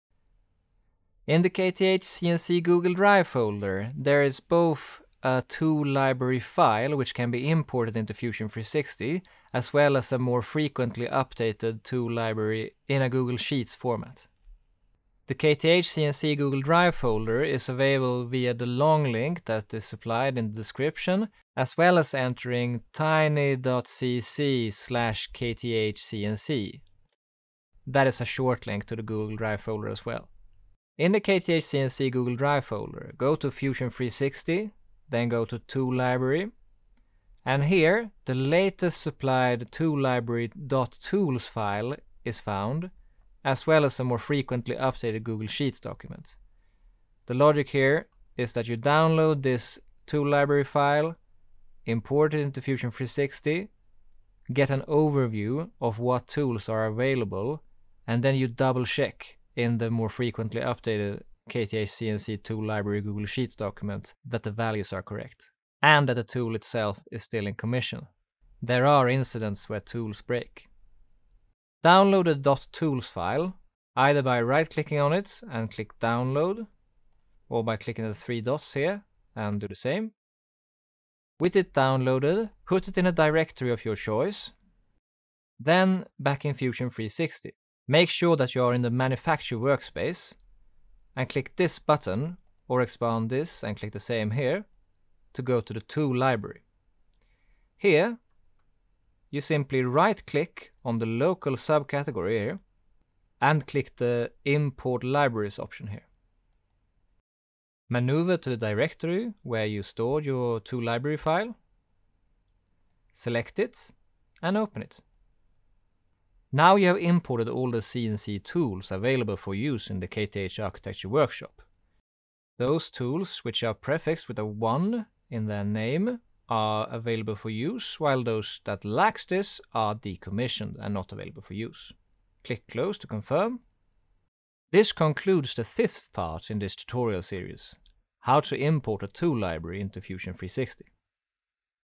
Tutorial – Importing a tool library into Fusion 360 (From 3D model to CNC fabrication, part 5)
This tutorial demonstrates how to import a tool library .tools file into Fusion 360, in the context of aiming to utilize the CNC router in the KTH Architecture school Workshop.

Timestamps:
0:00 Download the .tools Tool Library file from the KTH_CNC Google Drive
1:25 Import the Tool Library into Fusion 360
2:14 Outro

This tutorial is part of a series, "From 3D model to CNC fabrication":